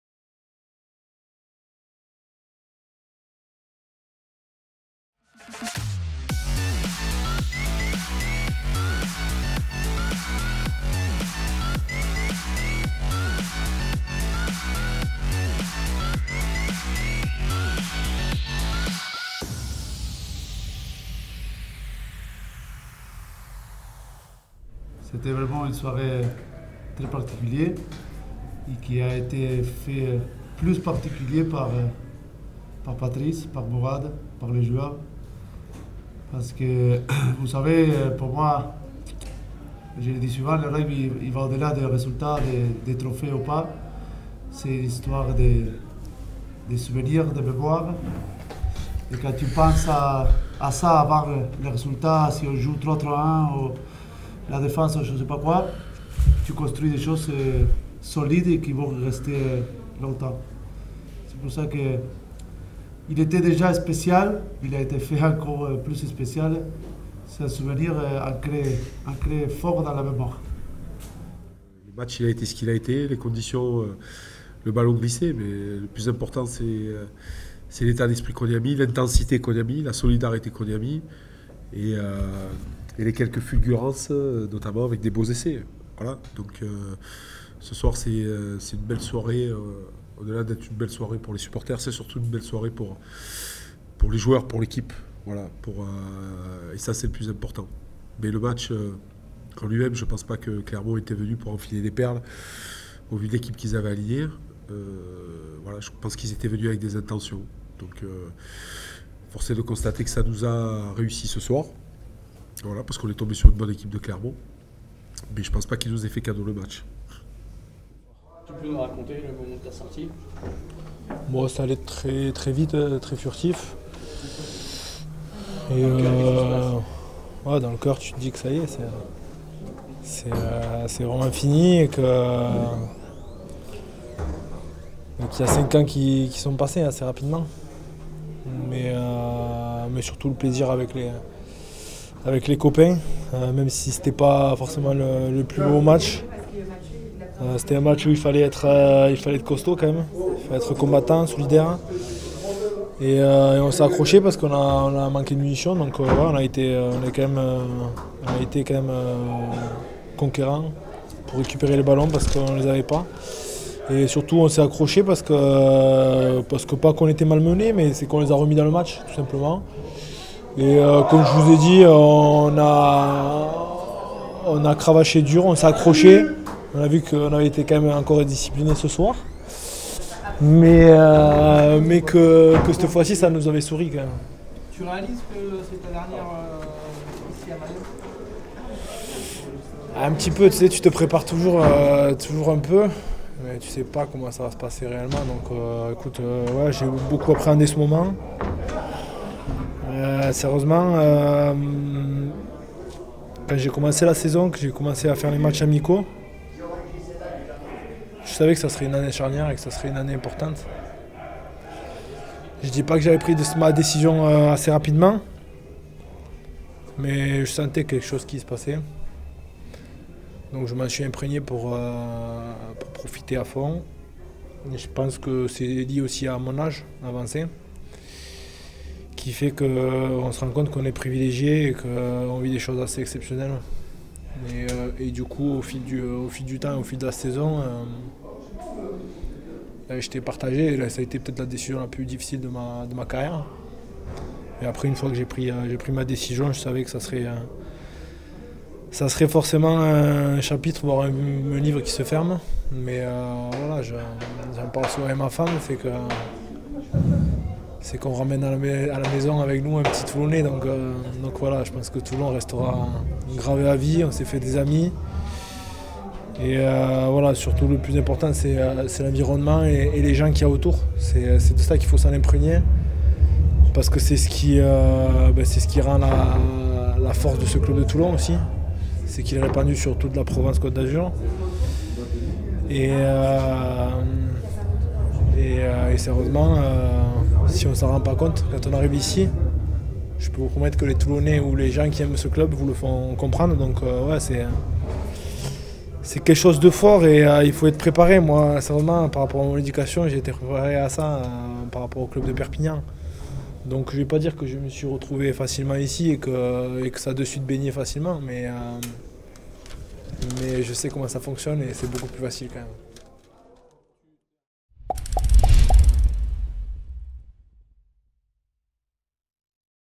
Conférence de presse d'après-match J25 Toulon-Clermont
Avec les interventions de Juan Martin Fernandez Lobbe, Patrice Collazo et Guilhem Guirado.